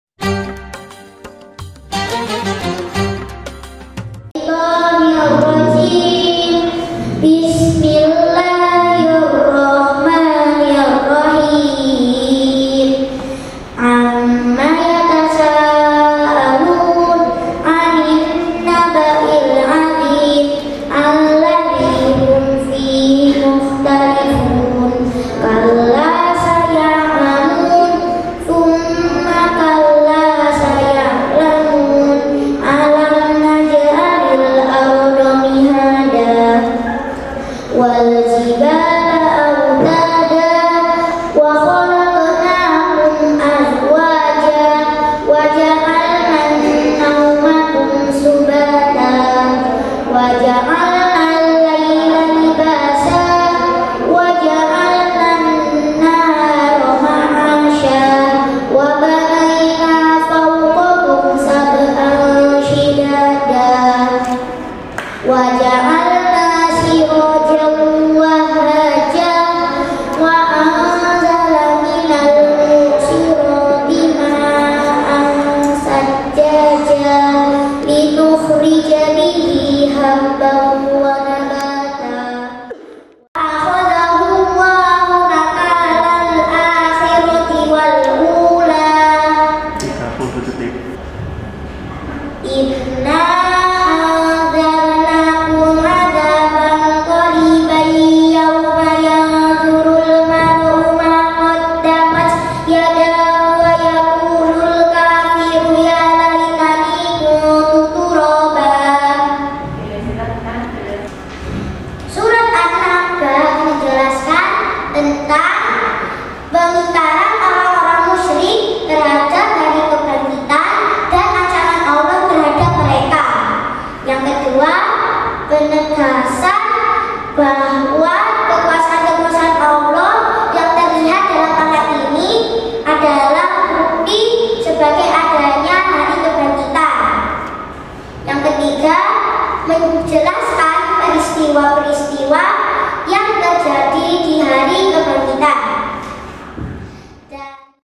FESTIVAL RAMADHAN BABAK FINAL | KELAS 4
Alhamdulillah, seluruh perlombaan babak final di Festival Ramadhan usai dilaksanakan. Semua santri telah menampilkan kemampuan mereka dengan maksimal. Tetap Semangat dan terus ukir prestasi.